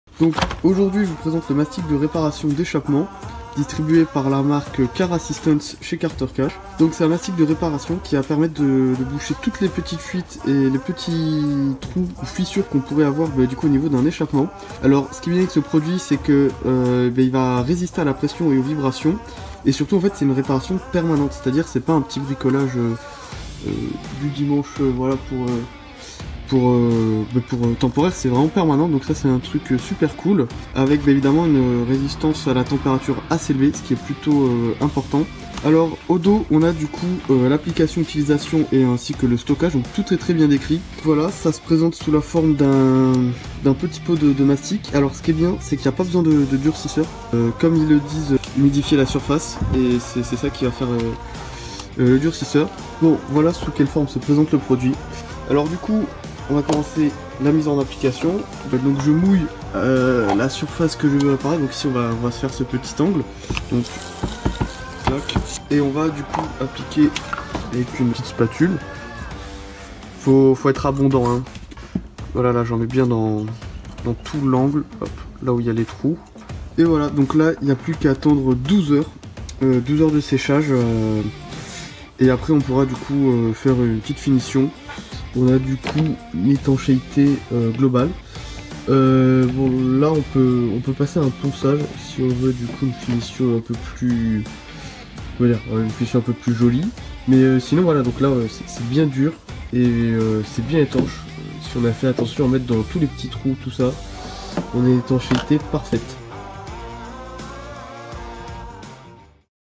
MASTIC REPARATION ECHAPPEMENT 200GR - Avis client de Thibault
Mastic réparation d'échappement 300 g MDD

- Permet de boucher les trous et fuites sur les systèmes d'échappement
- Résistant à la pression et aux vibrations
- Réduit les bruits
- Réparation permanente

Application

- Spécialement formulé pour la réparation du système d'échappement, dont le silencieux, les tuyaux d'échappement, les pots catalytiques, etc.
- Le mastic peut résister à des températures pouvant atteindre 1100°C.
- Il durcit par voie d'évaporation de l'eau, avant de durcir davantage par application d'une source de chaleur supplémentaire, ce qui garantit un joint parfaitement étanche à toutes les vapeurs nocives.